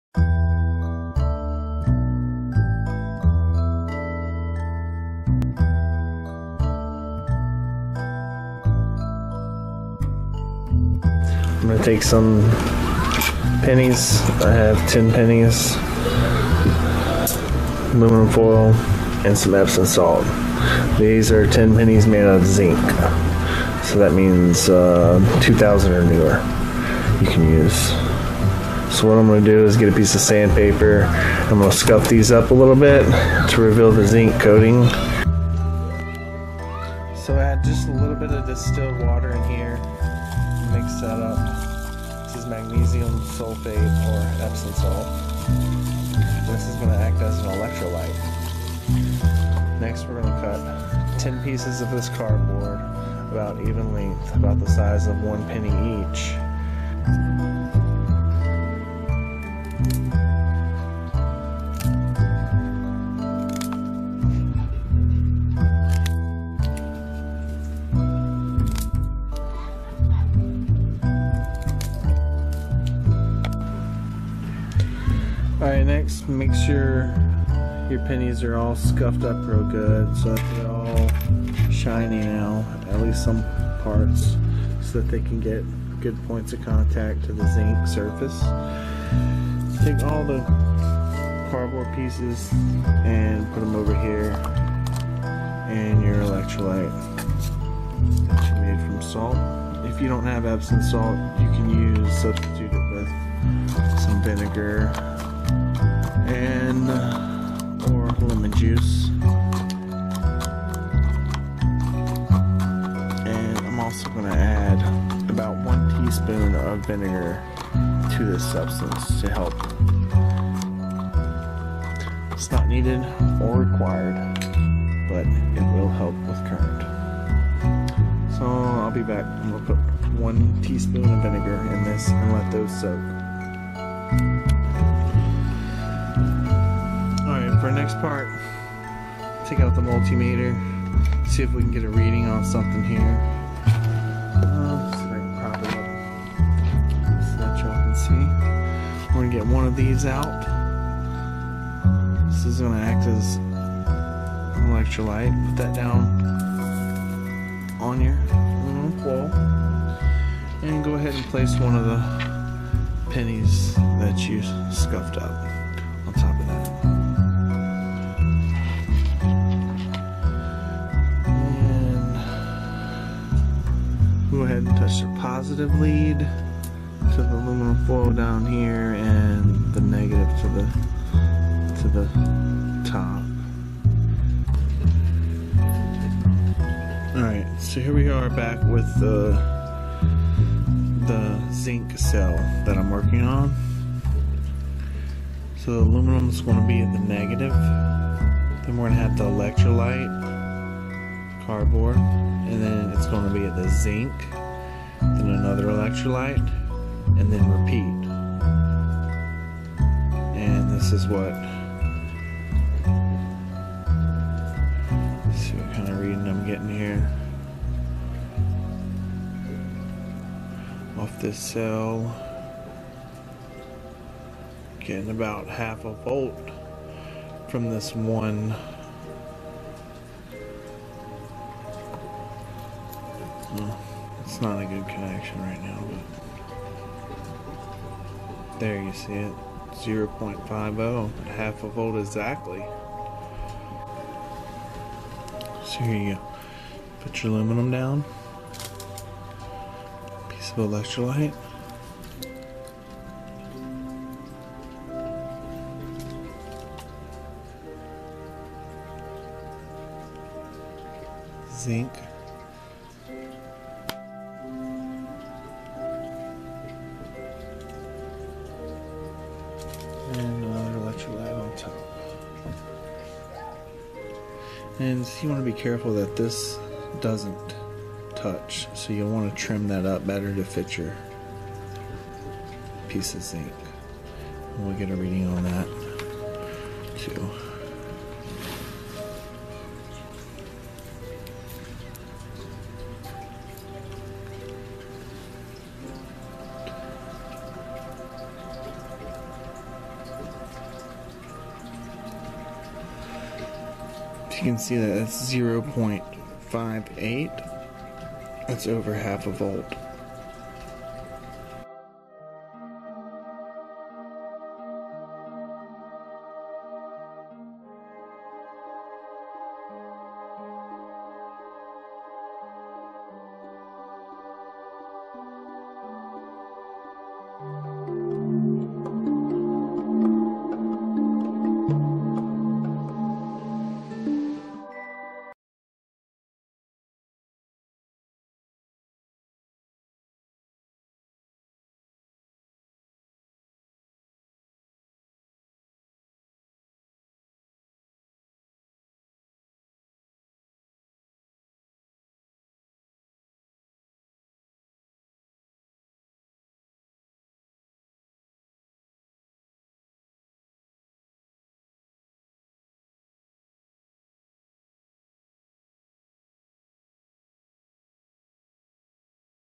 DIY How to Build Zinc Rechargable 10 cent Penny Battery Cells potentially Stronger then Li-Ion
Here is a Zinc Penny Battery Cell.  I show you how to make one too.  A great thing for anyone to know is how to produce their own energy.  Basically it only costs 10 cents.  Or you can use zinc washes if you have some in the toolbox.  Almost all washes are made out of zinc.  In any emergency situation it may become very useful to know how to make your own batteries and this can save you a lot of money if you have the time to make your own or just want to for fun.  Nothing new ever came from trying the same things over and over.  Seek search and find and share knowledge with the world.  Have a Blessed Day!